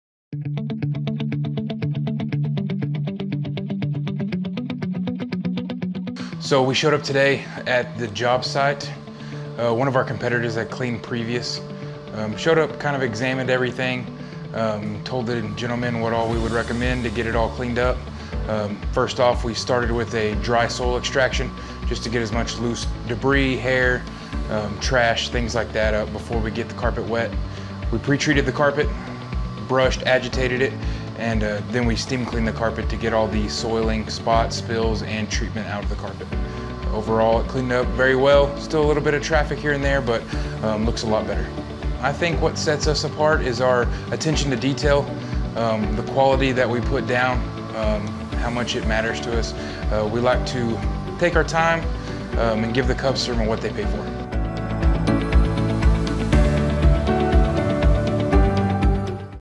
Keeping It Clean - Competitor Redo
Join Carpet Tech's Crew Chief, Cade Simmons, as he tackles a redo on a botched job from a local competitor. His crew shows why Carpet Tech's commitment to detail and client satisfaction is so important.